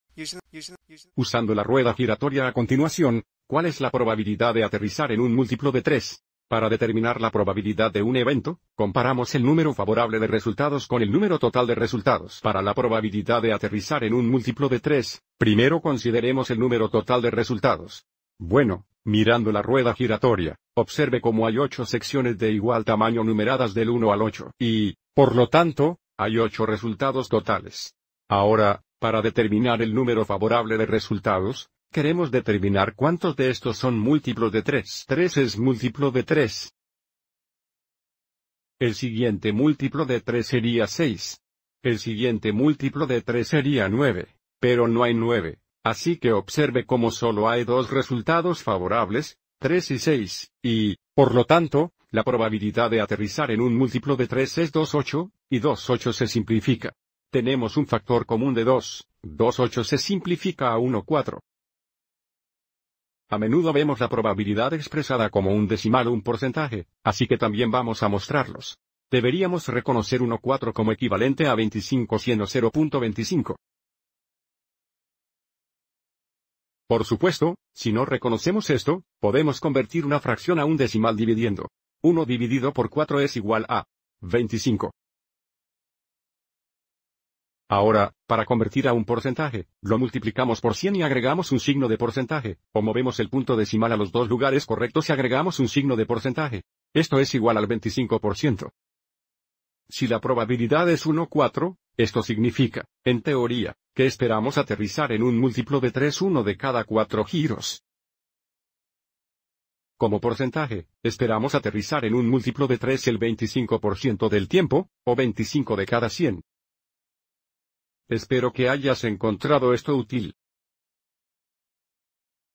Ej: Encuentra la probabilidad de aterrizar en un múltiplo de 3 usando una ruleta
Este video explica cómo determinar la probabilidad de aterrizar en un múltiplo 3 usando una ruleta. Sitio